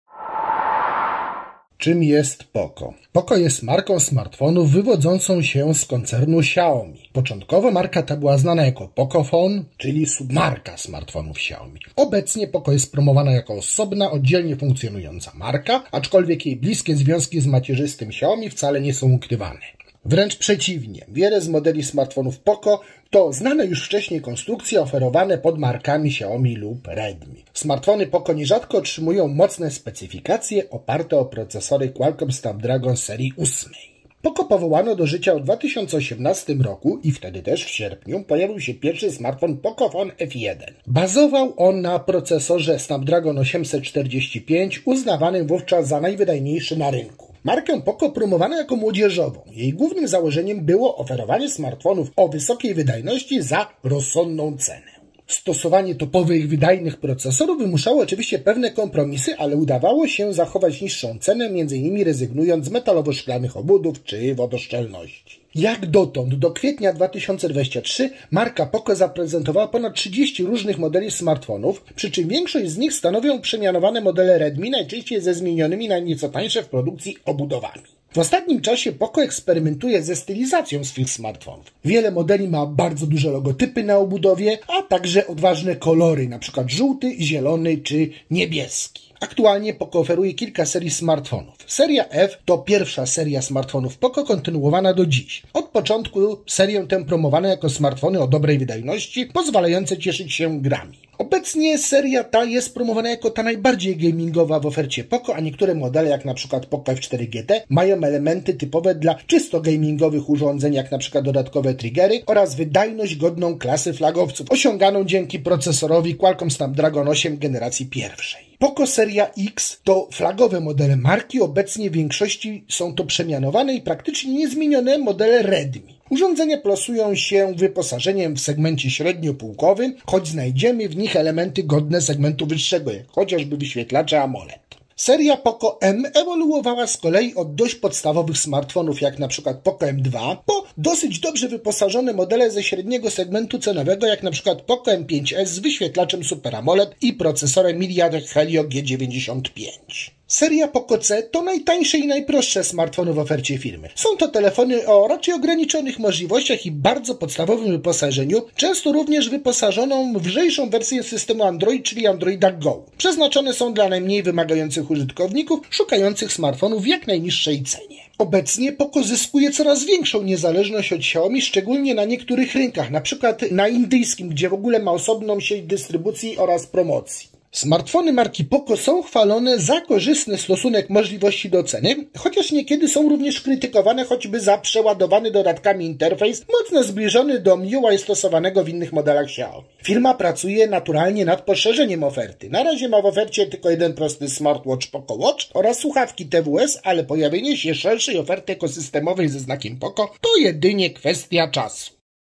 Poznaj telefony POCO!
POCO jest marką smartfonów wywodzącą się z koncernu Xiaomi. Początkowo marka ta była znana jako PocoPhone - submarka smartfonów Xiaomi. Obecnie POCO jest promowana jako osobna, oddzielnie funkcjonująca marka, aczkolwiek jej bliskie związki z macierzystym Xiaomi wcale nie są ukrywane. Wręcz przeciwnie - wiele z modeli smartfonów POCO to znane wcześniej konstrukcje oferowane pod markami Xiaomi lub Redmi. Smartfony nierzadko otrzymują mocne specyfikacje, oparte o procesory Qualcomm Snapdragon 8 Series.

POCO powołano do życia w 2018 roku, wtedy też - w sierpniu - pojawił się pierwszy smartfon Pocophone F1,  bazujący na procesorze Qualcomm Snapdragon 845, uznawanym wówczas za najwydajniejszy na rynku. Markę POCO promowano jako młodzieżową. Jej głównym założeniem było oferowanie smartfonów o wysokiej wydajności za rozsądną cenę. Stosowanie topowych, wydajnych procesorów wymuszało oczywiście pewne kompromisy, ale udawało się zachować niższą cenę m.in. rezygnując z metalowo-szklanych obudów czy wodoszczelności.

Marka POCO zaprezentowała dotąd (kwiecień 2023) ponad 30 różnych modeli smartfonów, przy czym większość z nich stanowią przemianowane modele Redmi, najczęściej ze zmienionymi na nieco tańsze w produkcji obudowami. W ostatnim czasie POCO eksperymentuje ze stylizacją swych smartfonów - wiele modeli ma bardzo duże logotypy marki na obudowie, a także odważne kolory, np. żółty, zielony czy niebieski.

Aktualnie, POCO oferuje kilka serii smartfonów:
- POCO F Series - Pierwsza seria smartfonów POCO, kontynuowana do dziś. Od początku serię promowano jako smartfony o dobrej wydajności, pozwalające cieszyć się grami. Obecnie seria ta jest promowana jako najbardziej gamingowa, a niektóre modele - jak np. POCO F4 GT mają elementy typowe dla czysto gamingowych urządzeń oraz wydajność godną klasy flagowców, osiąganądzięki procesorowi Qualcomm Snapdragon 8 Gen 1.
- POCO X Series - POCO z serii X to flagowe modele marki, obecnie w większości są to przemianowane i praktycznie nie zmienione smartfony Redmi. Urządzenia plasują się wyposażeniem w segmencie średniopółkowym, choć znajdziemy w nich elementy godne segmentu wyższego, jak chociażby wyświetlacze AMOLED.
- POCO M Series - Seria M ewoluowała od dość podstawowych smartfonów (jak np. POCO M2), po dość dobrze wyposażone modele ze średniego segmentu cenowego, jak np. POCO M5s z wyświetlaczem Super AMOLED i procesorem MediaTek Helio G95.
- POCO C Series - Seria C to najtańsze i najprostsze smartfony w ofercie POCO. Są to telefony o ograniczonych możliwościach i bardzo podstawowym wyposażeniu, często wyposażone w lżejszą wersję systemu, czyli Android Go. Przeznaczone dla najmniej wymagających użytkowników, poszukujących smartfonów w jak najniższych cenach.

Obecnie POCO zyskuje coraz większą niezależność od Xiaomi, szczególnie na niektórych rynkach - np. w Indiach, gdzie ma osobną sieć dystrybucji oraz promocję. Smartfony marki POCO są chwalone za korzystny stosunek możliwości do ceny, choć niekiedy są krytykowane za przeładowany dodatkami interfejs, mocno zbliżony do MIUI stosowanego w innych modelach Xiaomi. Firma pracuje też nad poszerzeniem oferty - na razie ma w ofercie tylko jeden prosty smartwatch (POCO Watch) oraz słuchawki TWS, ale pojawienie się szerszej oferty ekosystemowej ze znakiem POCO jest jedynie kwestią czasu.